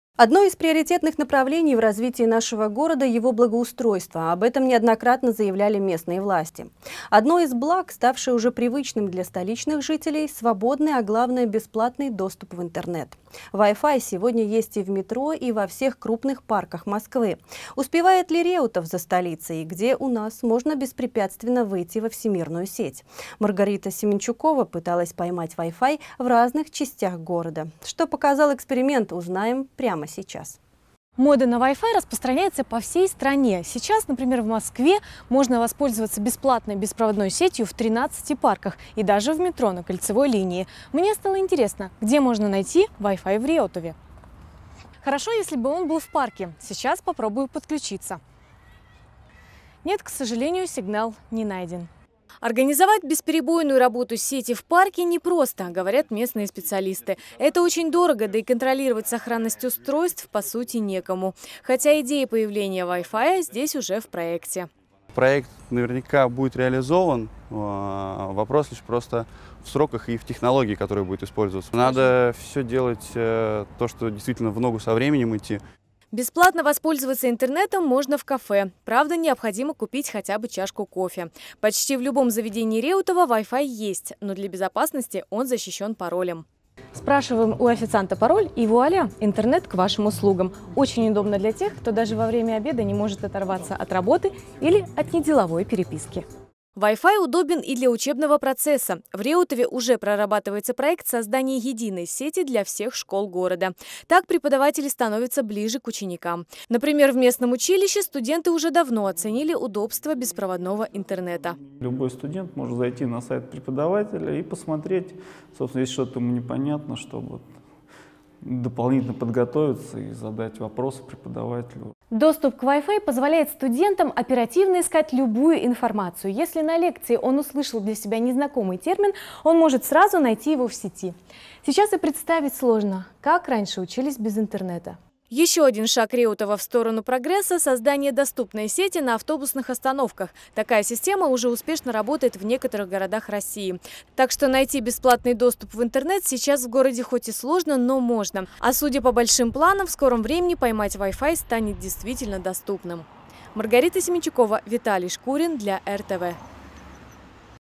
Где можно найти зоны бесплатного вайфая в Реутове 20.08.13
Одно из приоритетных направлений в развитии нашего города -- его благоустройство. Об этом неоднократно заявляли местные власти.  Одно из благ, ставшее уже привычным для столичных жителей -- свободный, а главное бесплатный, доступ в интернет. Вай-фай сегодня есть и в метро, и во всех крупных парках Москвы. Успевает ли Реутов за столицей, и где у нас можно беспрепятственно выйти во всемирную сеть.